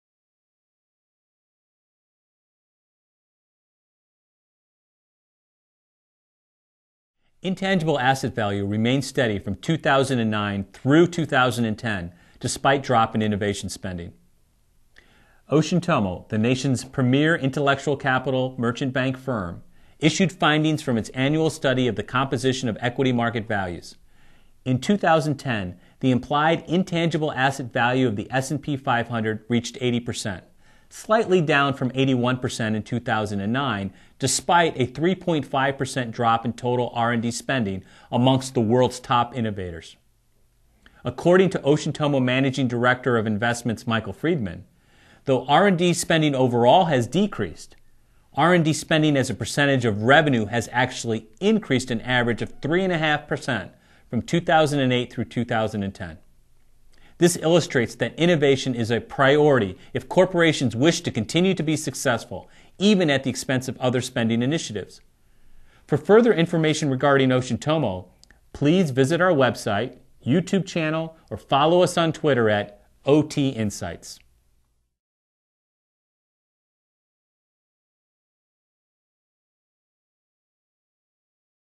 Ocean Tomo 2010 Intangible Asset Market Value Study Results
Intangible Asset Value Remains Steady from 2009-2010, Despite Drop in Innovation Spending.

Within the last quarter century, the market value of the S&P 500 companies has deviated greatly from their book value. This "value gap" indicates that physical and financial accountable assets reflected on a company's balance sheet comprises less than 20% of the true value of the average firm. Our further research shows that a significant portion of this intangible value is represented by patented technology.